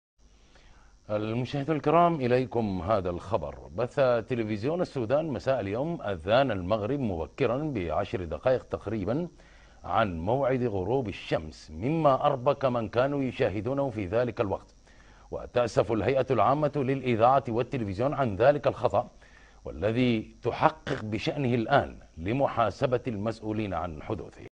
‏التلفزيون السوداني يبث أذان المغرب قبل موعده بـ 10 دقائق .. والجهات الرسمية تعتذر وتعد بالتحقيق
ولاتنسى تفعل التنبيهات عشان يوصلك كل جديد شطحات